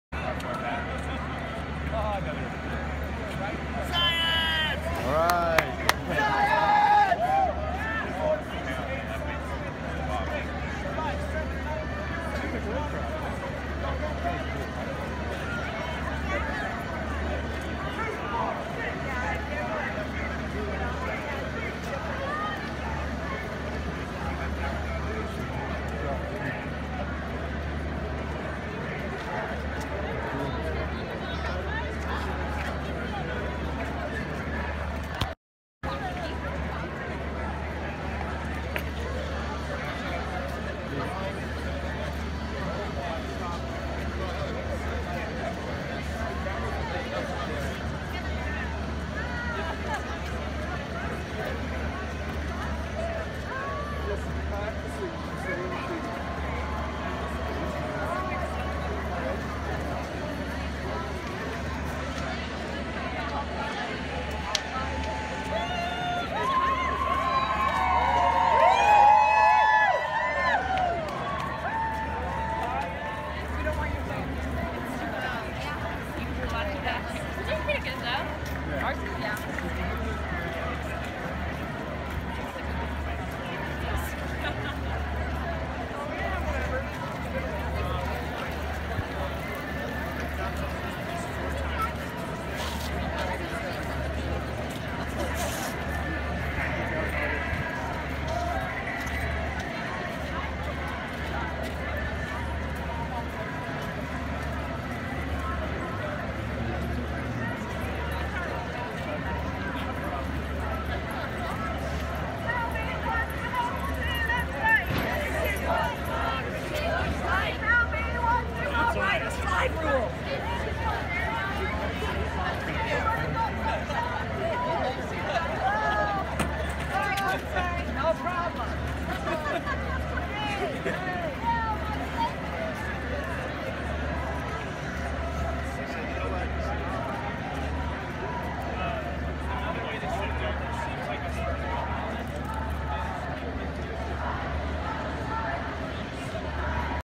Earth Day Science March - Denver, CO 4/22/2017
Check out the scientist in the first few moments of this video! makes me smile real big! This is footage from the Earth Day Science March in Denver Colorado on April 22nd 2017. I had such a fun day canvassing for Greenpeace this day! Hey All Climate change is real, wake up and start taking your own personal actions towards individual responsibility of sustainable living. I'm not there yet myself but I am working towards it.